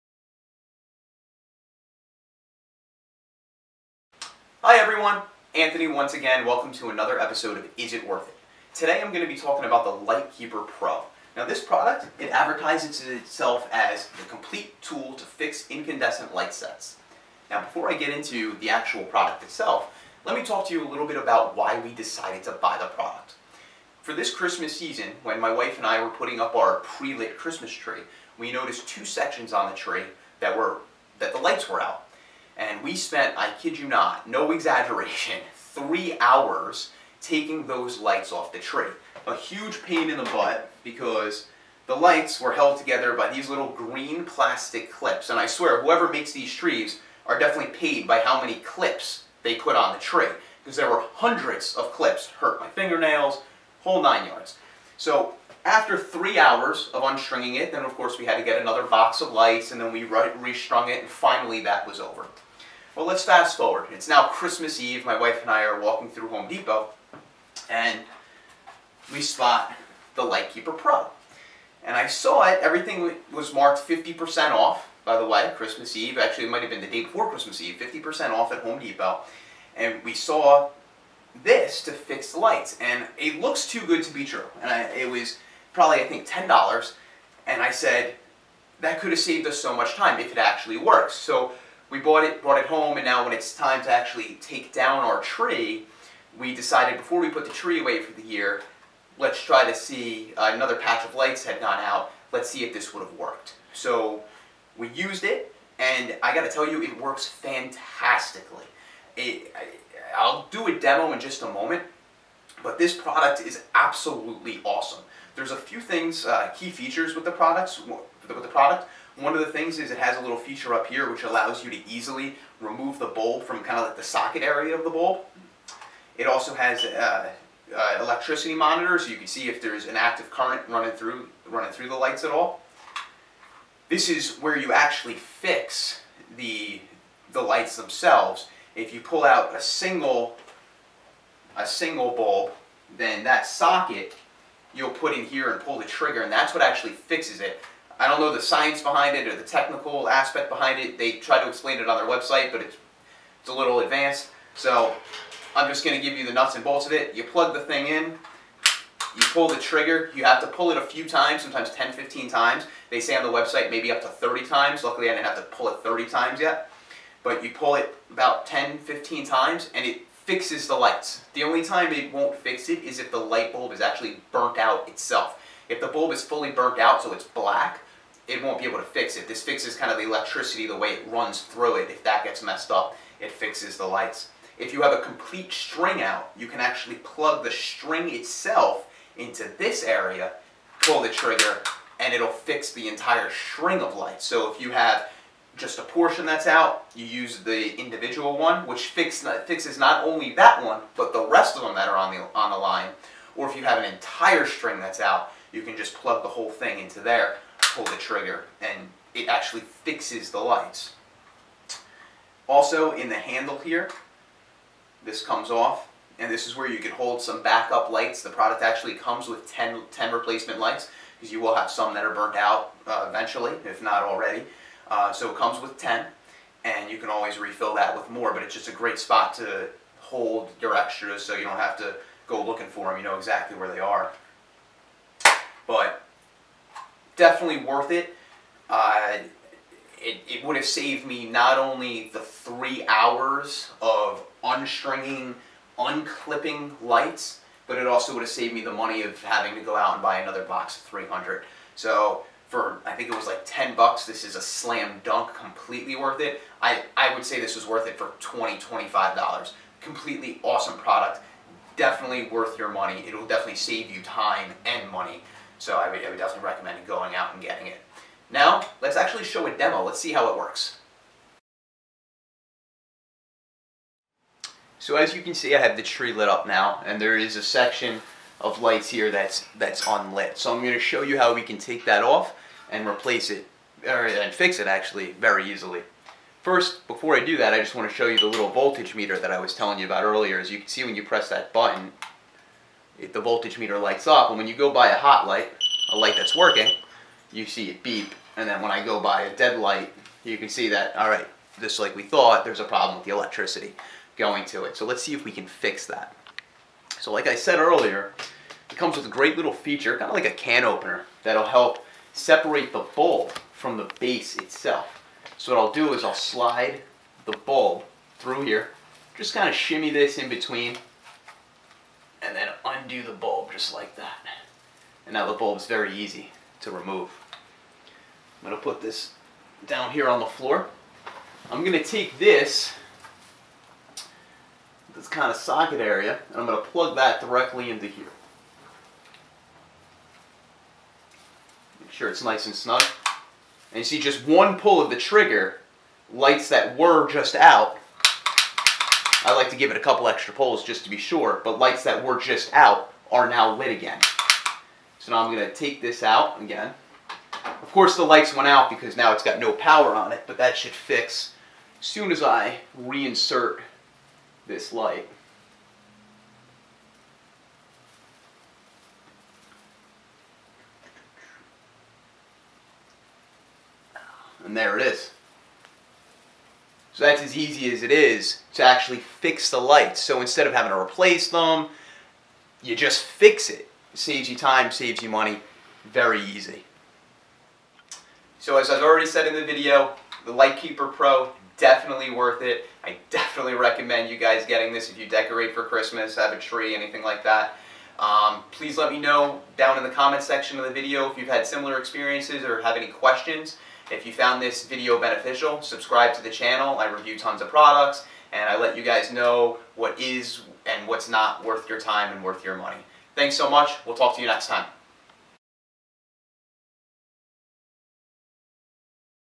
Light Keeper Pro Review.. Is it worth it?
Here is the "Is It Worth It" review of the light keeper pro.. Check out the video to see if we recommend this product..